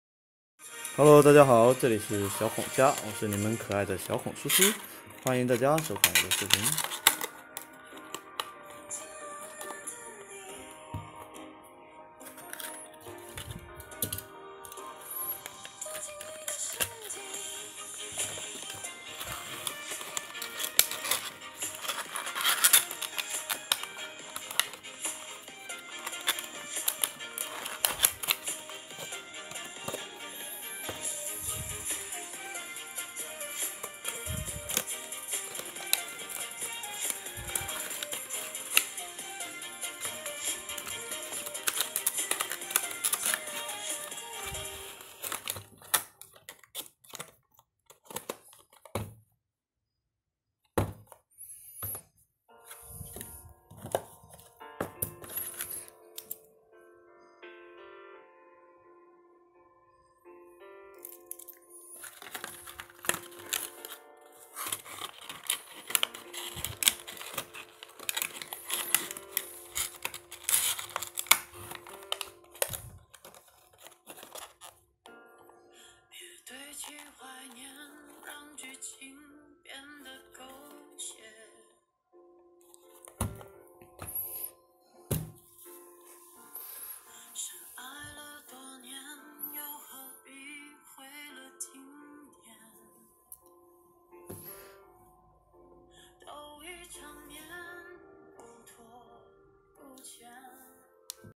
Kids toys, alloy forklift, alloy excavator, snow truck engineering vehicle unboxing video
Nursery rhymes and learning color color baby play toy entertainment nursery rhymes